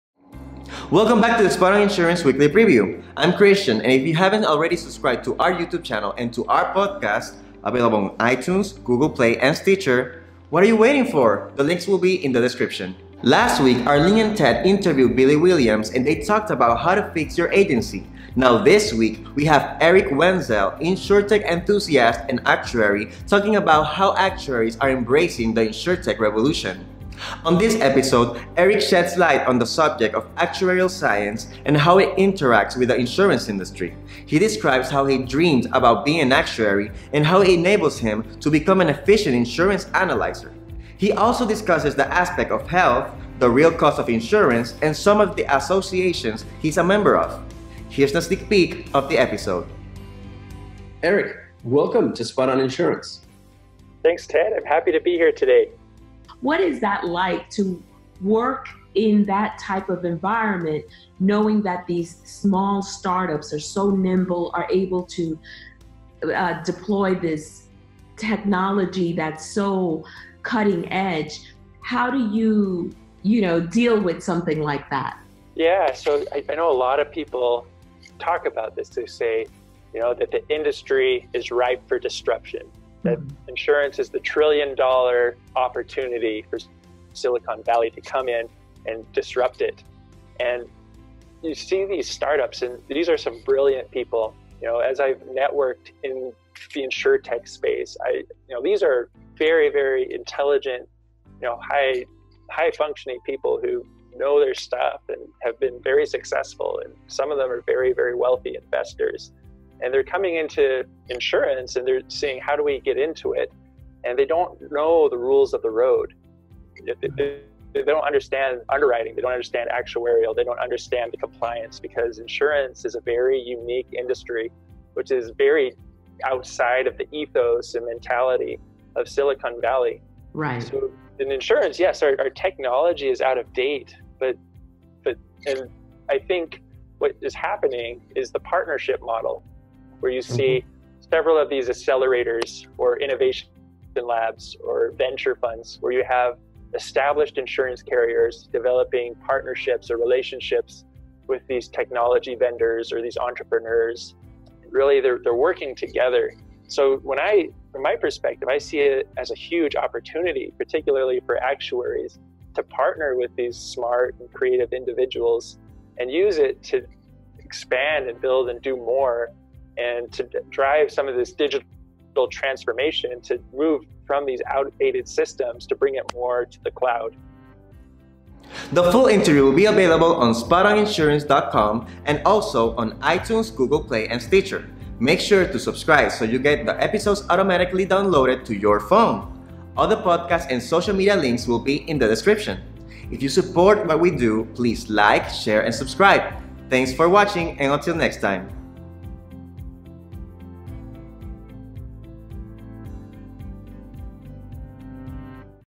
EMBRACING THE INSURTECH REVOLUTION - WEEKLY PREVIEW EP. 94
Erik Wenzel is an InsurTech enthusiast and actuary. He describes himself as someone who has the brain of an actuary and passion of a Silicon Valley entrepreneur, and firmly believes the actuary profession is more than capable of leveraging opportunities presented by InsurTech. He currently works at CNA Insurance, a commercial property and insurance company that provides a range of insurance services for different sectors including construction, financial institutions, and healthcare.

Erik joins us today to shed light on the subject of actuarial science and how it interacts with the insurance industry. He describes how he dreamed about being an actuary and how it enables him to become an efficient insurance analyzer. He discusses the aspect of health, the real cost of insurance, and some of the associations he’s a member of. He also explains why he chose LinkedIn as his primary platform when it comes to sharing knowledge about the actuary profession.